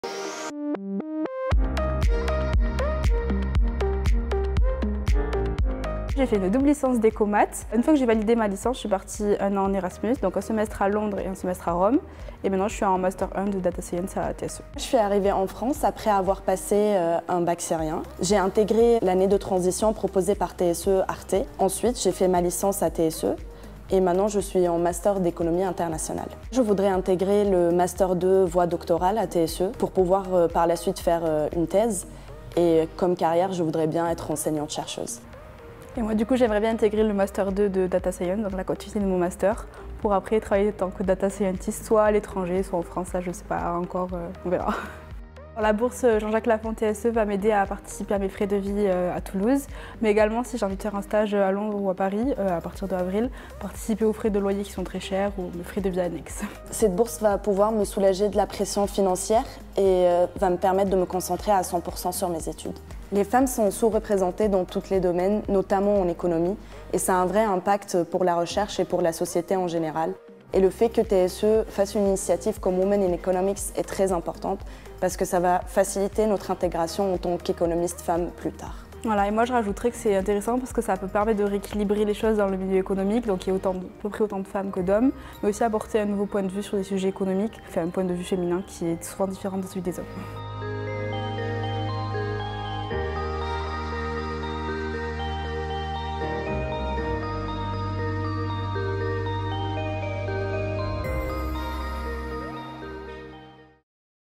Fondation Jean-Jacques Laffont-TSE scholarship : two holders feedback
Two holders of the FJJL-TSE scholarship share their experience of the scholarship.